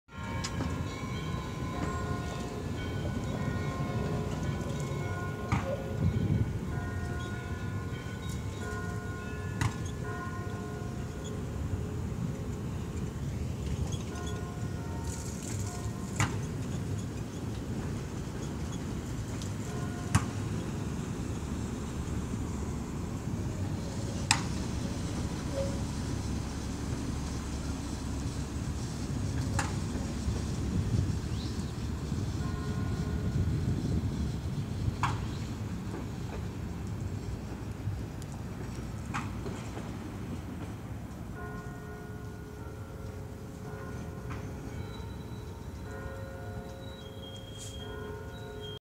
"отправление гуськом" Харьковские трамваи Tatra-T3SUCS:3067 6го маршрута и сцеп 589+590 23 маршрута
"отправление гуськом" Харьковские трамваи Tatra-T3SUCS #3067 6го маршрута и сцеп Tatra-T3SUCS 589+590 23 маршрута кольцо 602 мкрн